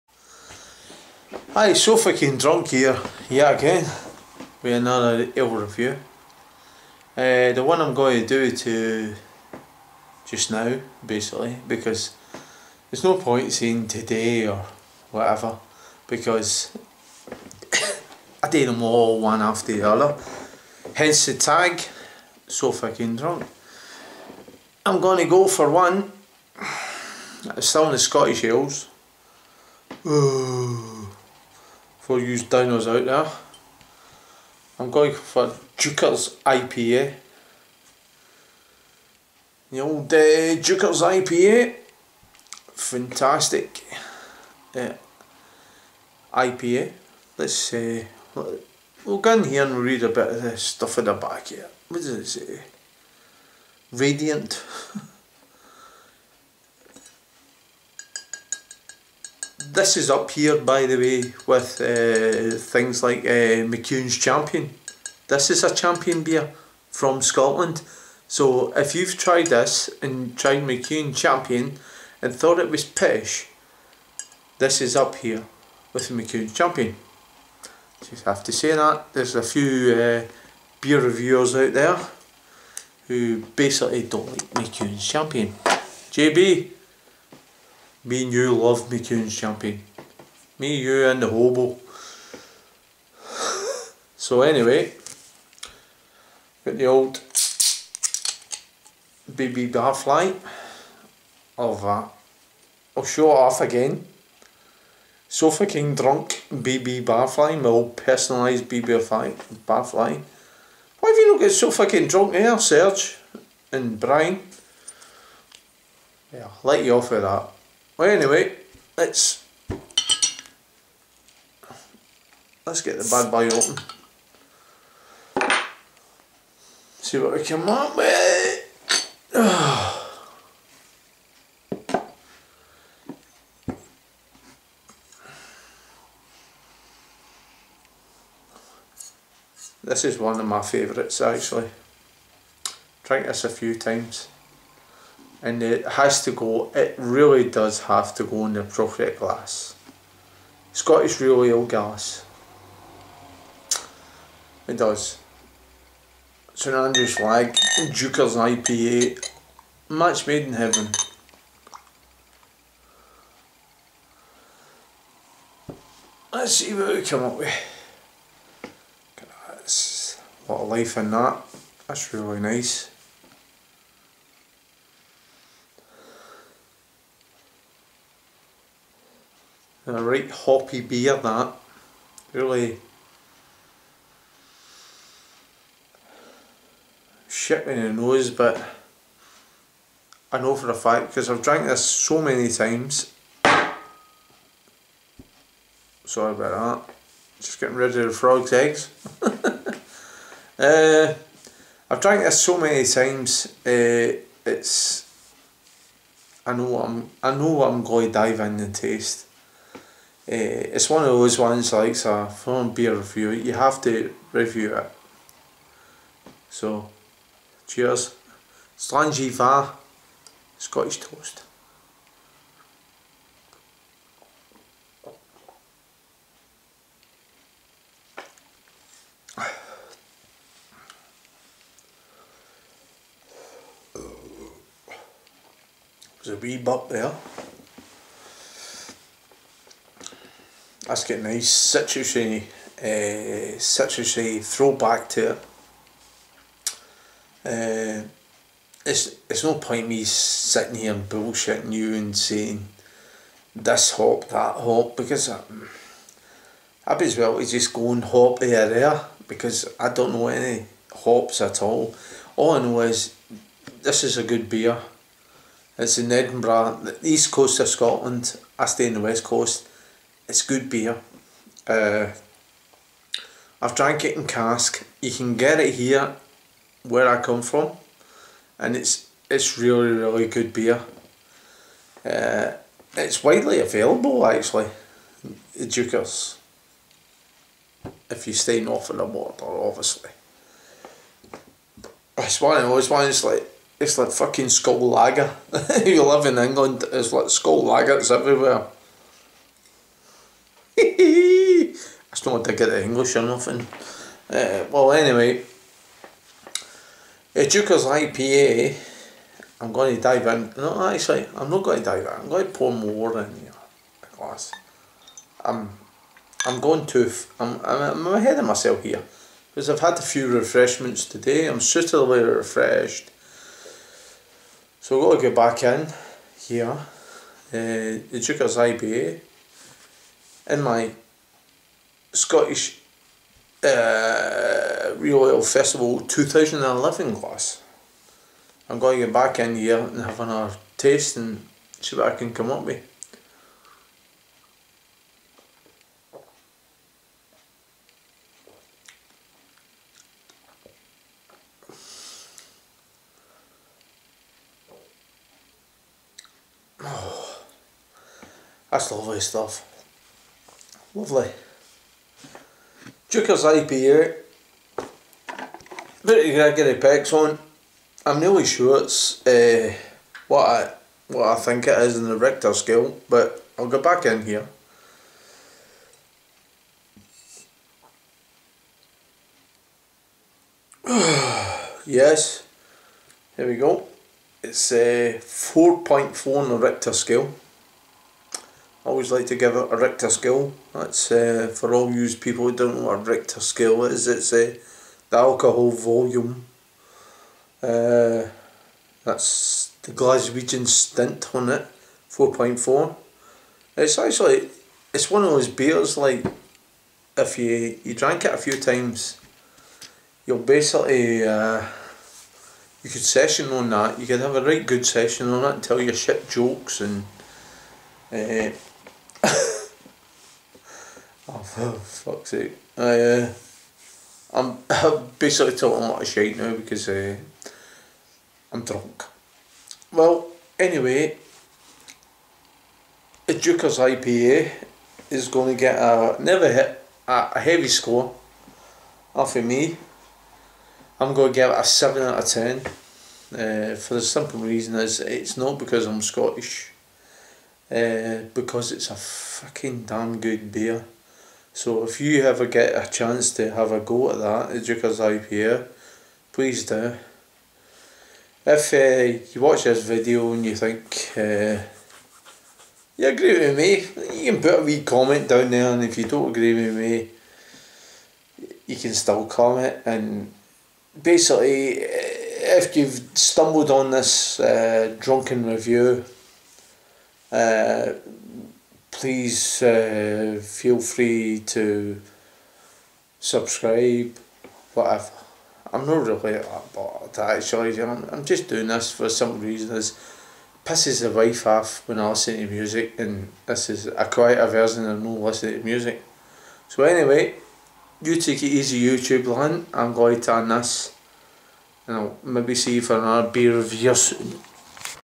#29 Caledonian Brewery Deuchars IPA 4.4% SCOTLAND
Beer review of the Classic Deuchars IPA by Caledonian Brewery SKD Rating 7-10